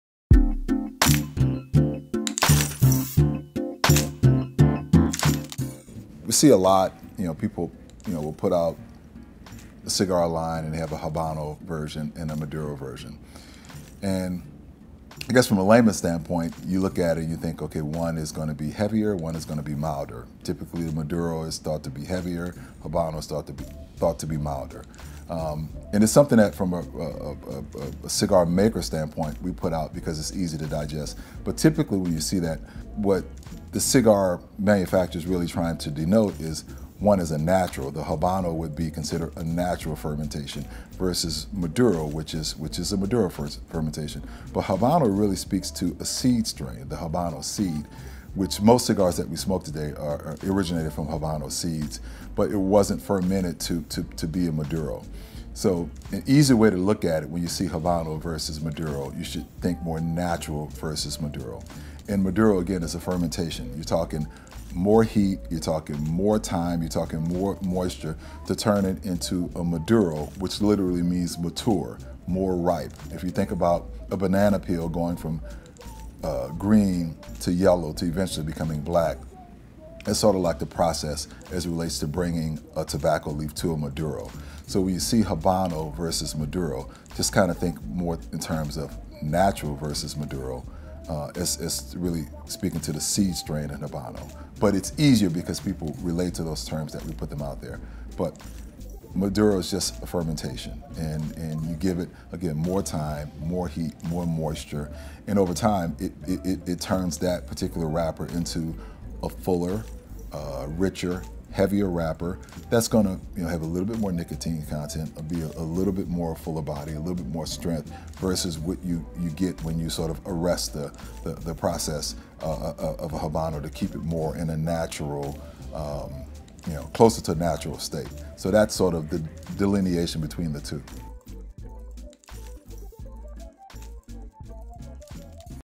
What is the Difference between Habano and Maduro Cigars?
Learn about the difference between habano and maduro cigars. Sean Williams of Cohiba explains the differences in the cigar types and blends. @CohibaCigars